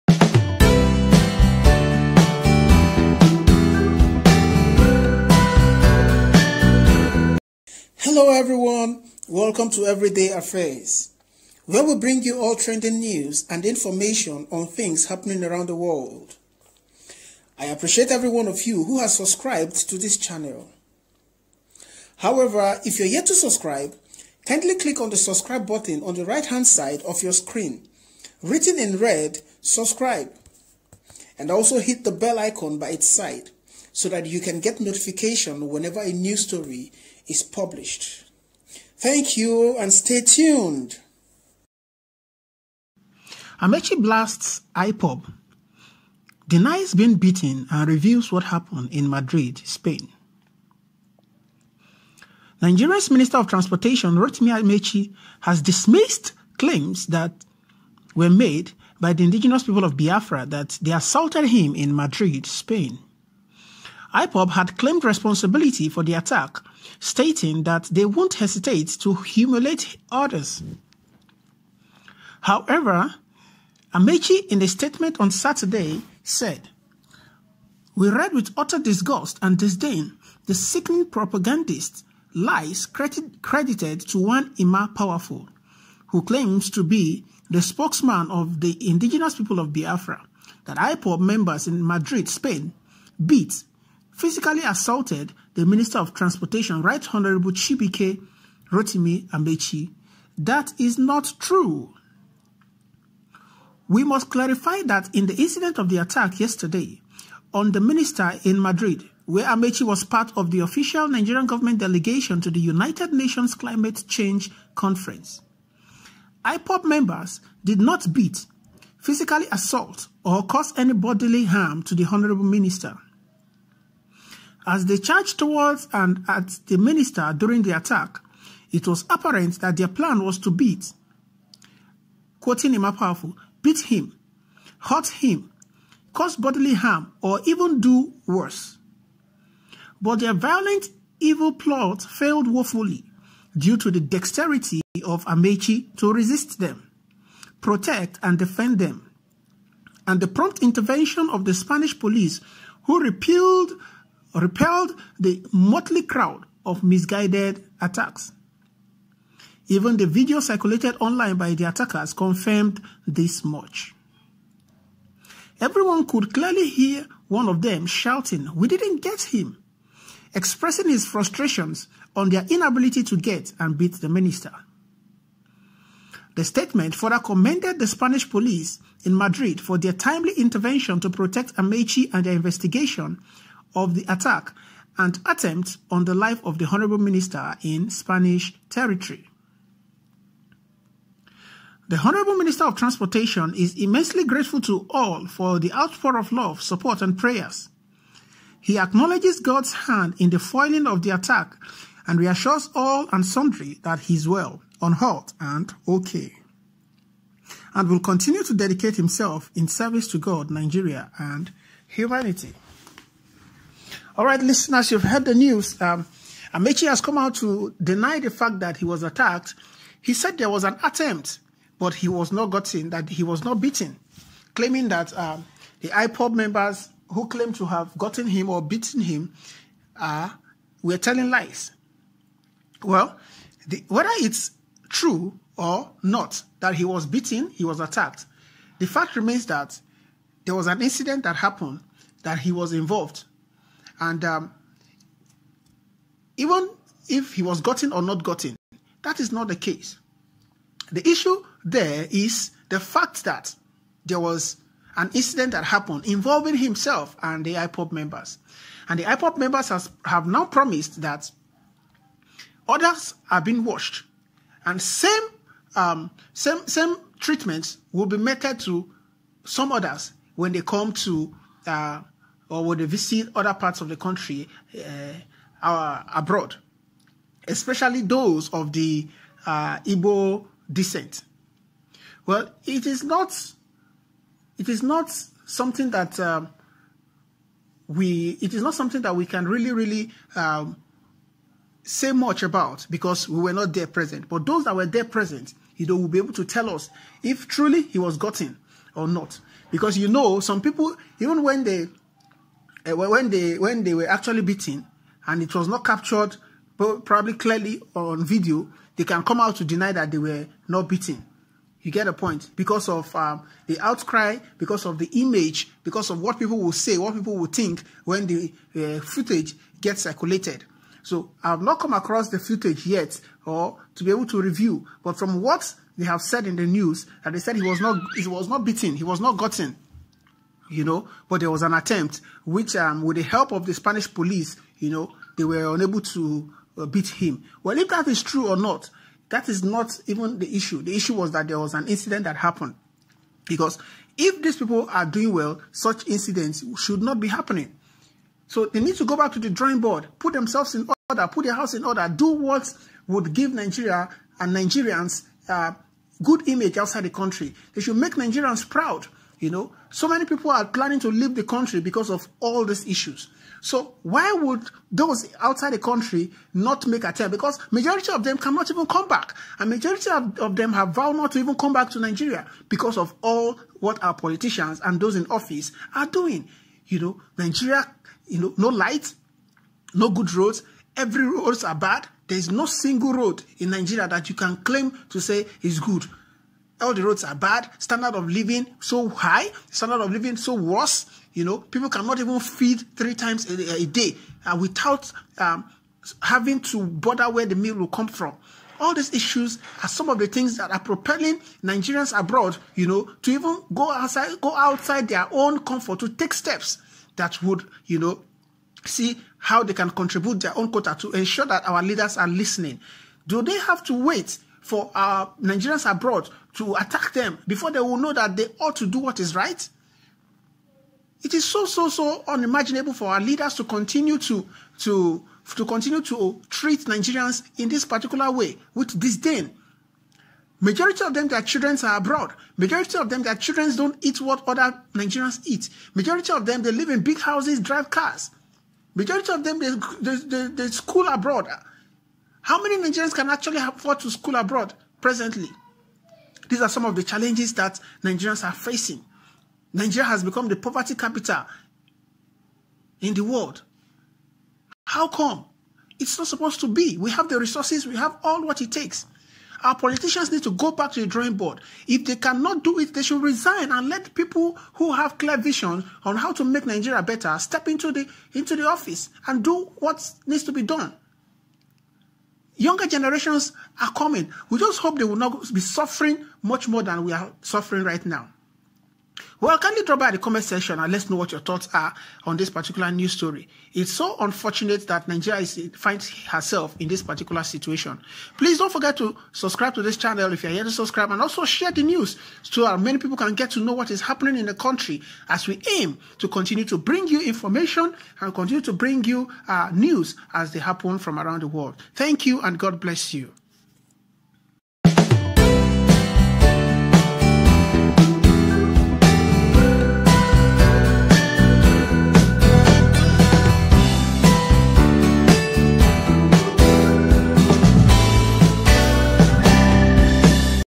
AMAECHI DENIES BEING BEATEN BY IPOB MEMBERS IN SPAIN
Amaechi denies being beaten in Spain by Ipob members.